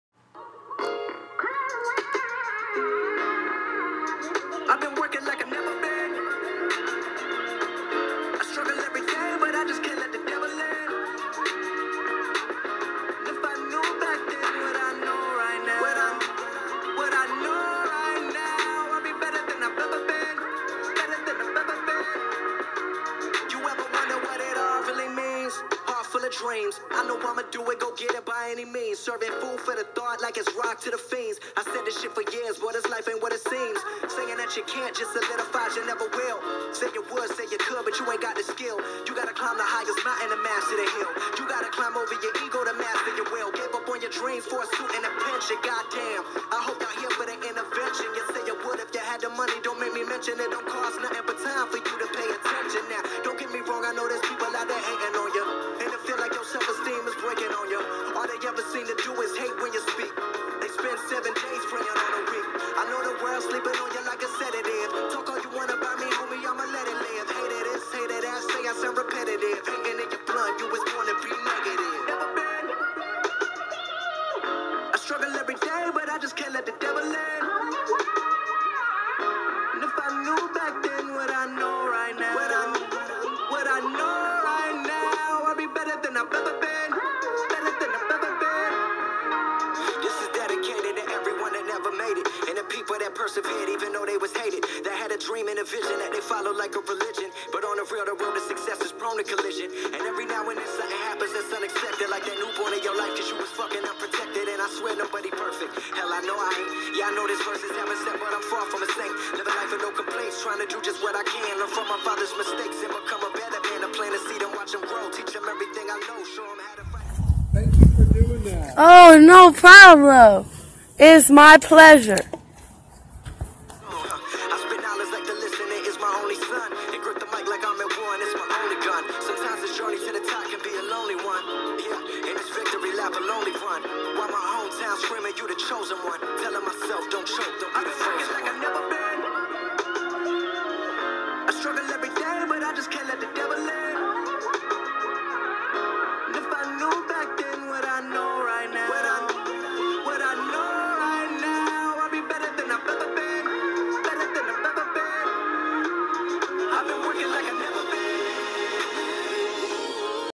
Life Is Beautiful
Beautiful sunrise at Garden of the Gods in Colorado Springs, CO. Along with a quick clean up while I was there.